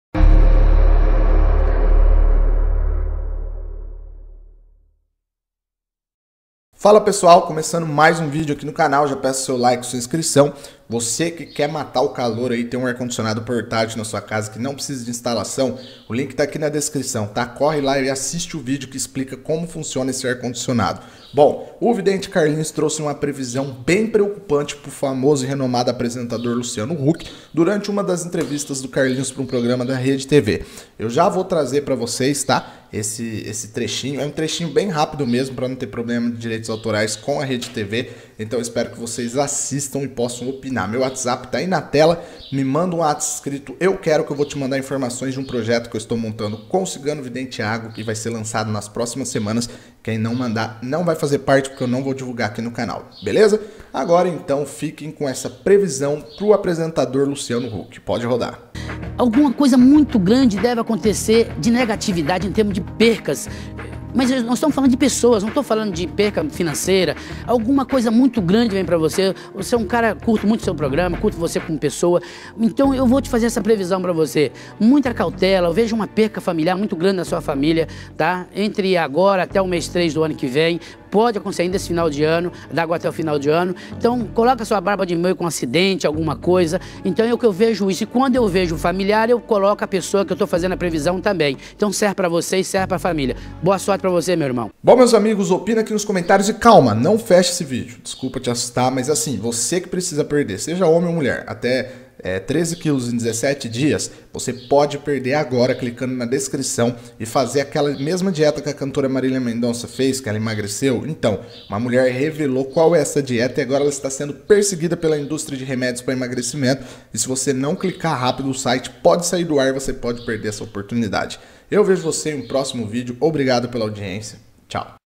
Vidente Carlinhos traz novas previsões e revela acidente para o apresentador Luciano Huck!
Seja um membro do QDR e receba conteúdos exclusivos do canal e tenha até mesmo lives exclusivas, fotos e muito mais!